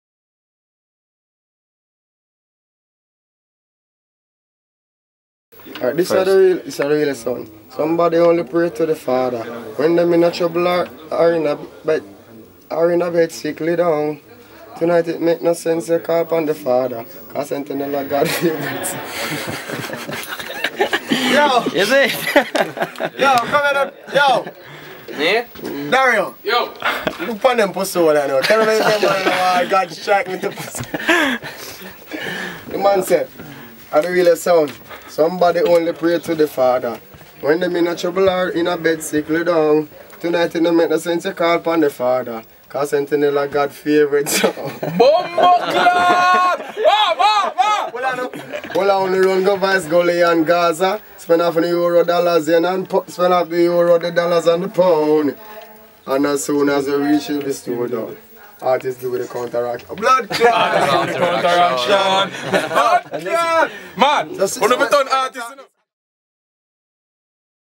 Sentinel Meets Konshens
Outtake from Konshens Dubplate Session for Sentinel Sound, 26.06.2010